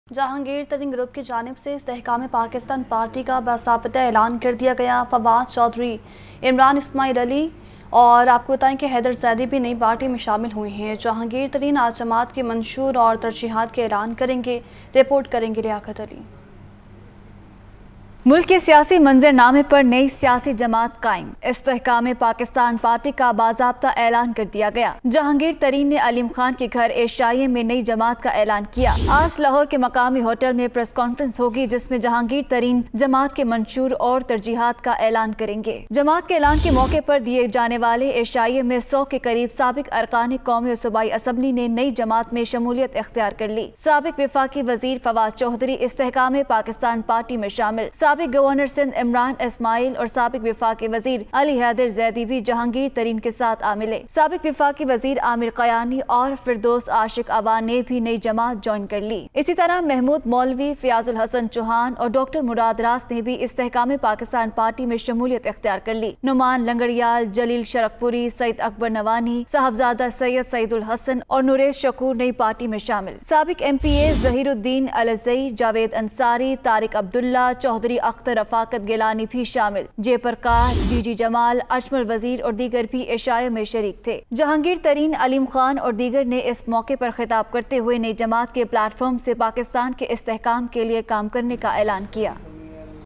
Watch | Fawad Ch Nay Kon Si New Party Join Kar Li? | Lahore News HD
PTI disgruntled leader Jahangir Tareen gained momentum in politics following the massive defections in the party, name for his new political party was unveiled on Wednesday, sources said.

Sources said that his party's name would be "Pakistan Istehkam Party" (PIP). Tareen would officialy announce the name of his party tomorrow.

Earlier, in an apparently major victory for Mr Tareen, PTI deserter Murad Raas joined his group. Mr Raas called on Mr Tareen accompanied by Abdul Aleem Khan and Awn Chaudhry. Mr Rass had bid adieu to the PTI along with other politicians including Hashim Dogar and others.

A day ago, three members of PTI’s “Hashim Dogar group”, including Raja Yawar Kamal, Mamoon Tarar, and Rai Aslam, joined Mr Tareen’s political camp.

It came as Mr Tareen had formed a three-member committee, including Awn Chaudhry, Sardar Tanvir Ilyas and Ishaq Khakwani, to rope in defectors as he is set to launch a new political party.

The committee decided to contact important figures from KP, including former party president Parvez Khattak and others. Former PTI senior vice president Fawad Chaudhry and Firdous Ashiq Awan had already held meetings with Mr Tareen.